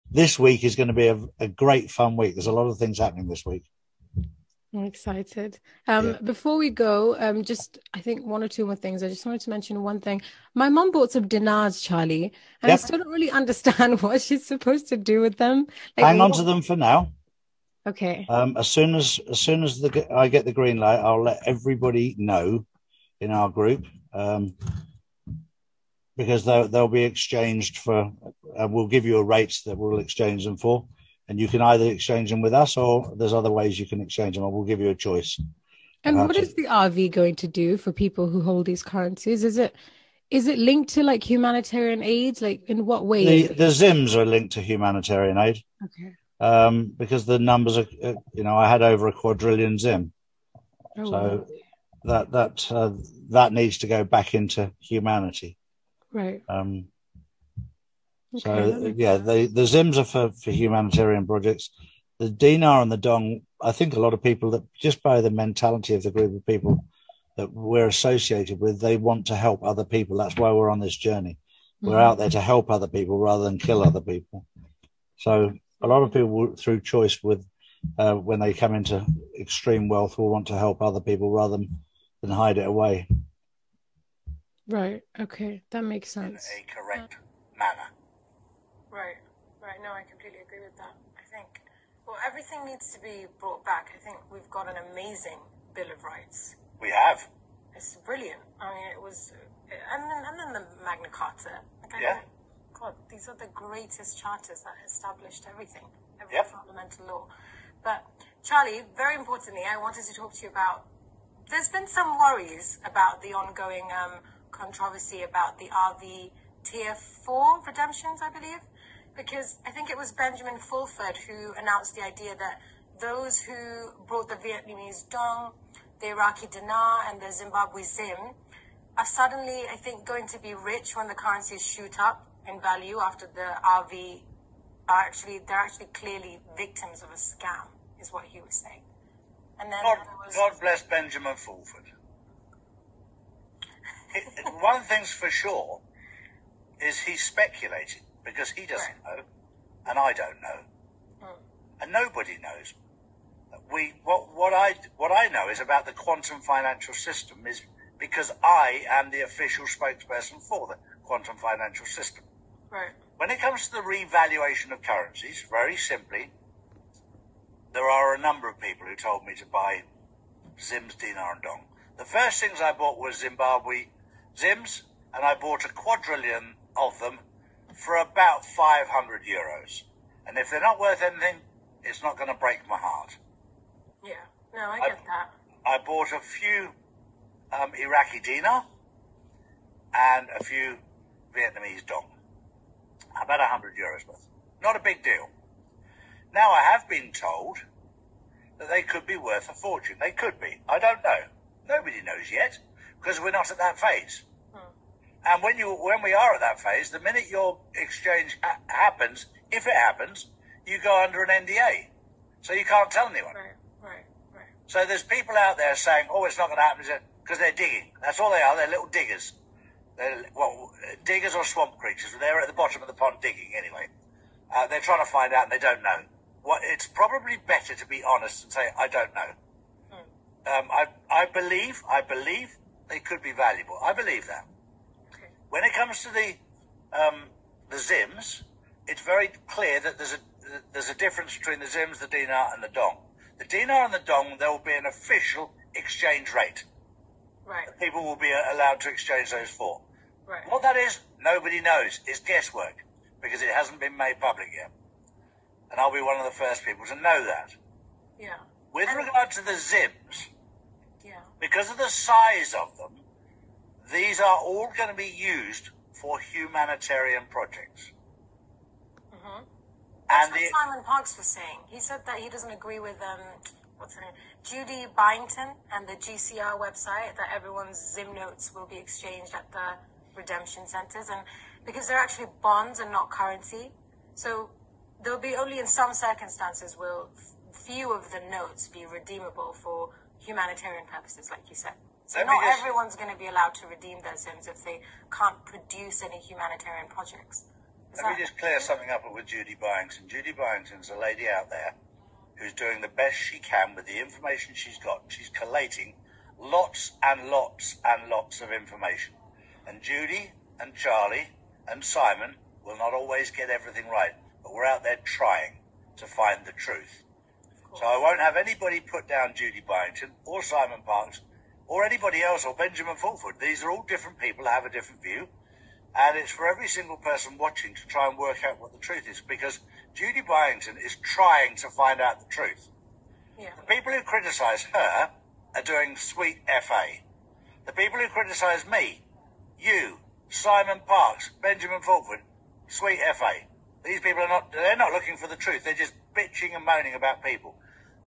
Charlie Ward Forgets What He Said To Tiana
Charlie Ward Forgets what he told Tiana Islam, 12 Months ago. Watch his narrative change and Tiana just swallows his lies.